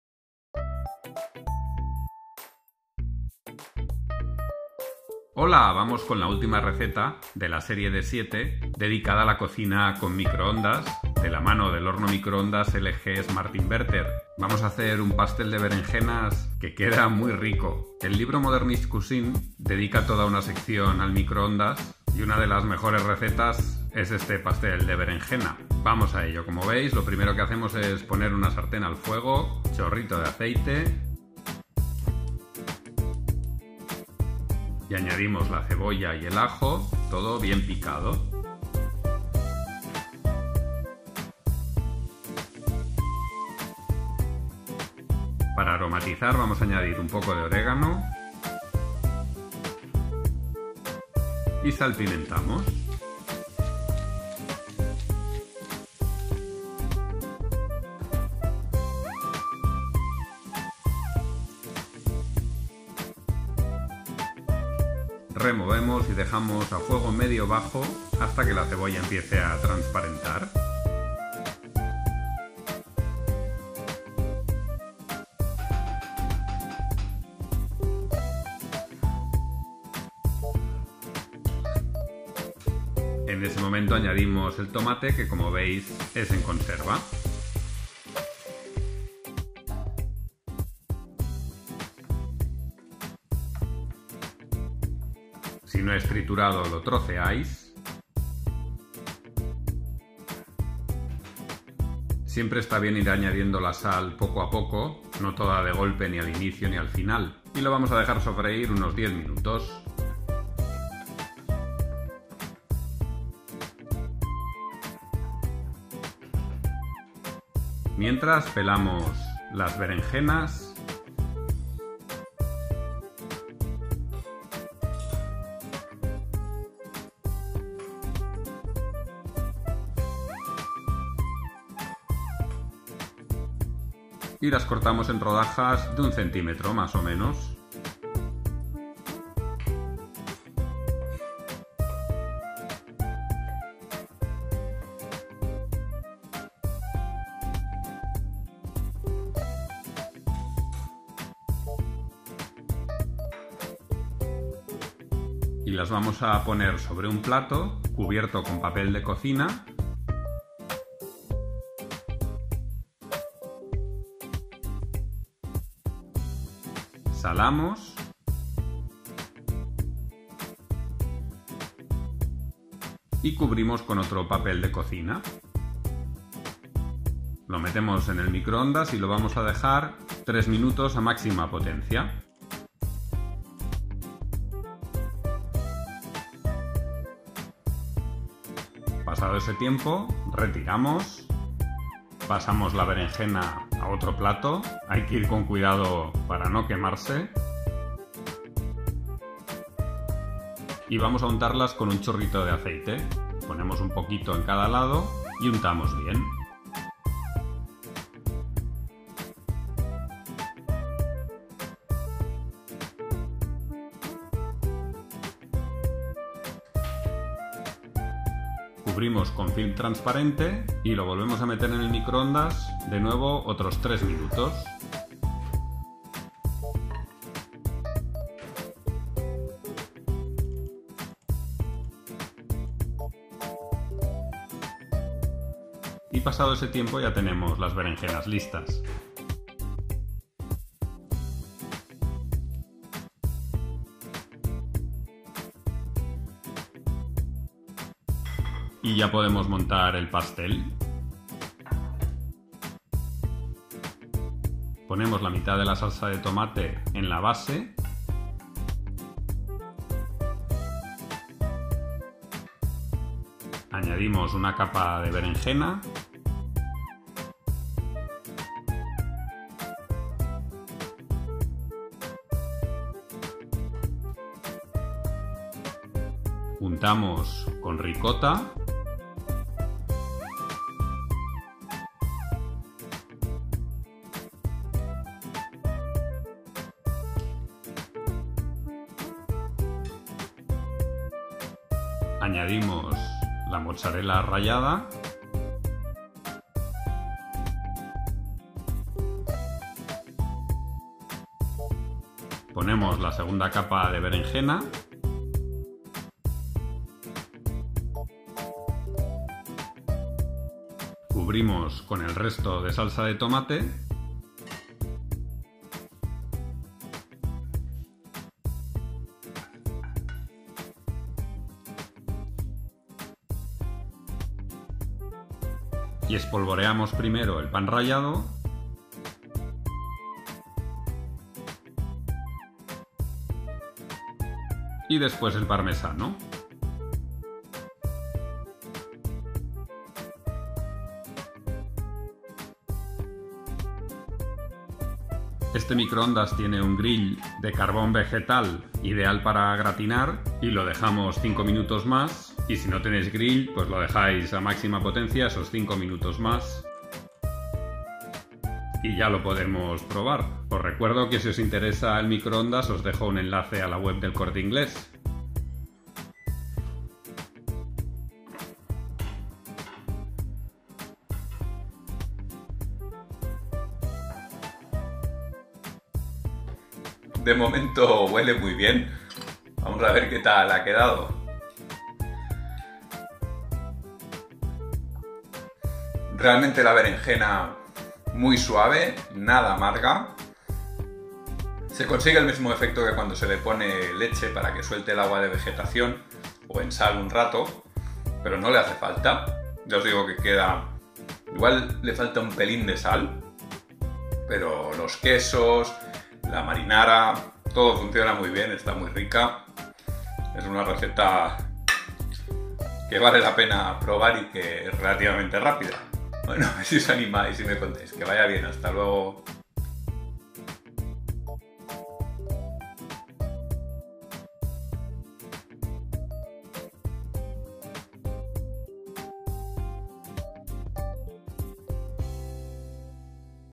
Cómo hacer berenjena a la parmesana al microondas
Nueva receta dedicada a la cocina con microondas. Estas berenjenas a la parmesana están espectaculares. A ver si os animáis.

Ingredientes:
3 berenjenas
400 g de tomates
1 cebolla
4 dientes de ajo
Orégano
150 g de ricota
150 g de mozzarella
50 g de pan rallado
50 g de parmesano
Aceite de oliva virgen extra
Pimienta negra
Sal